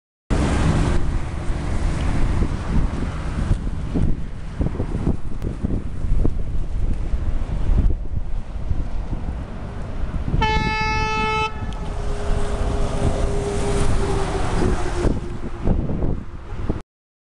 UTS444 605 SERVICIO METROTREN DESTINO RANCAGUA POR PK 100
PK 100 - PASO PEATONAL PICHIGUAO
2 DE DICIEMBRE DEL 2013

OBSERVACIONES: ESTE VIDEO FUE REALIZADO A 40 HORAS DE LA TRAGEDIA